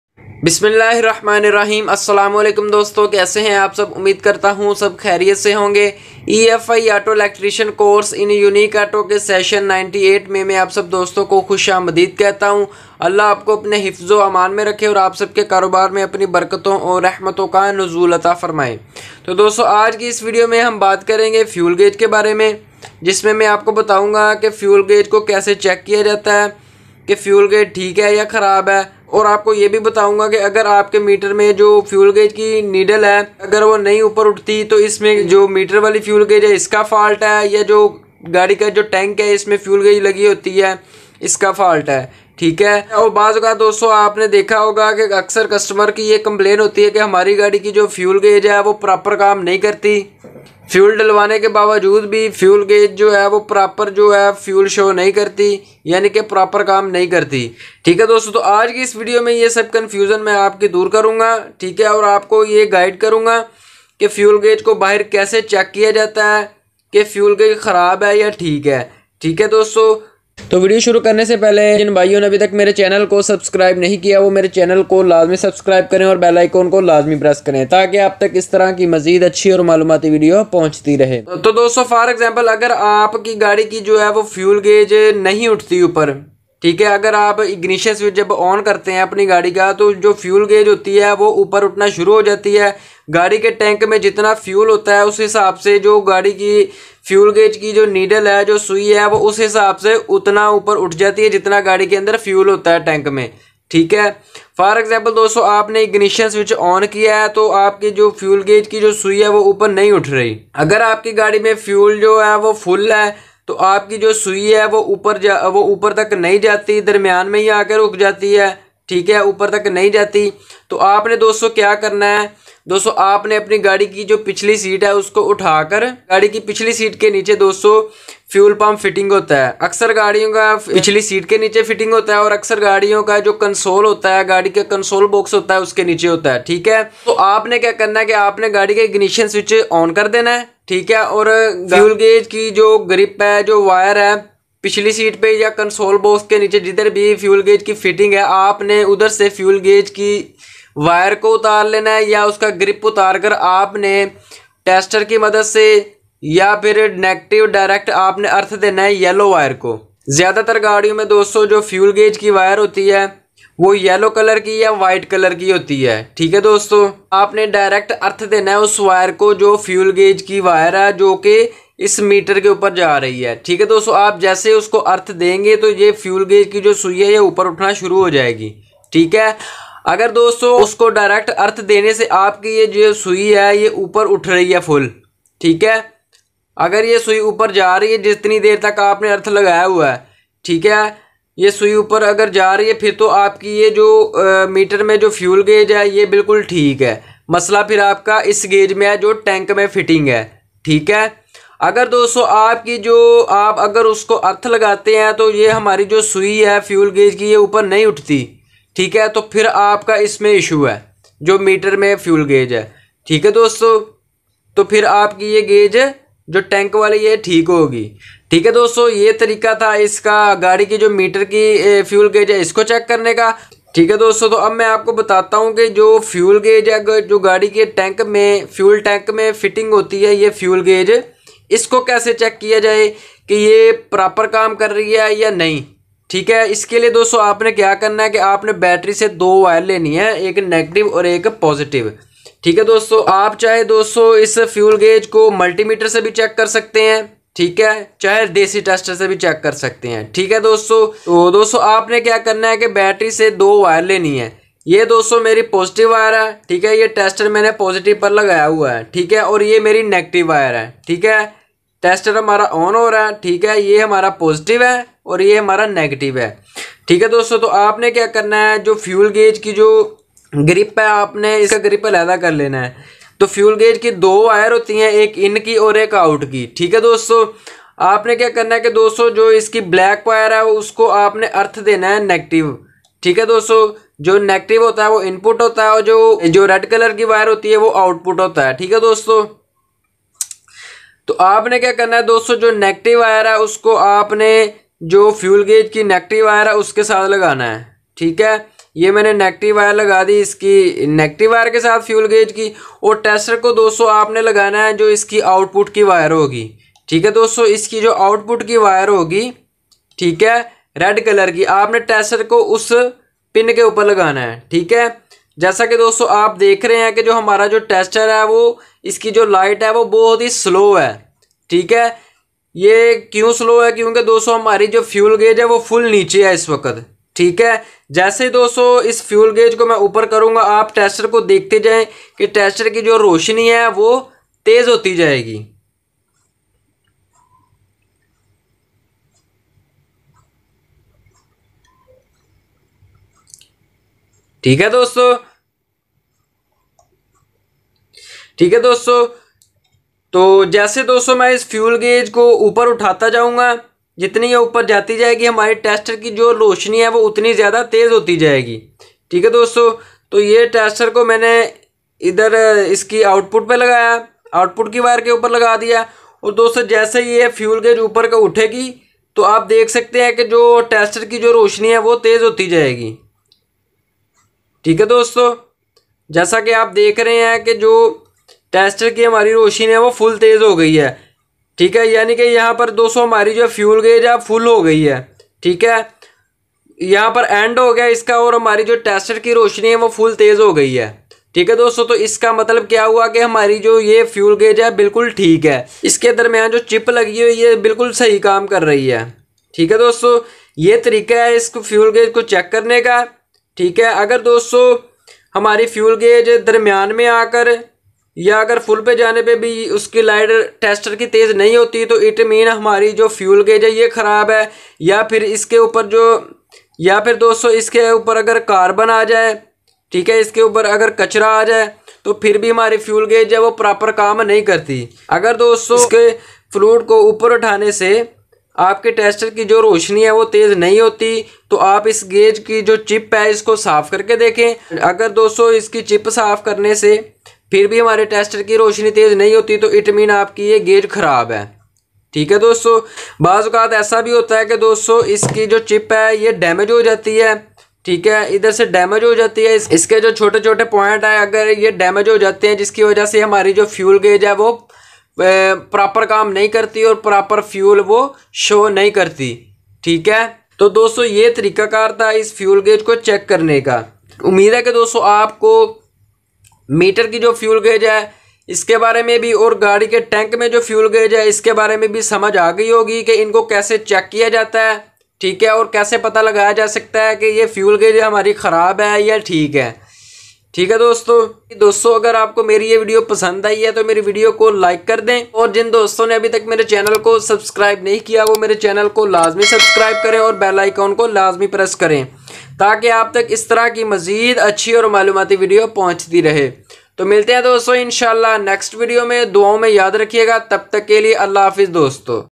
How to test and repair car fuel gauge | fuel gauge meter problem | fuel gauge troubleshooting
efi auto electrician course in unique auto session 98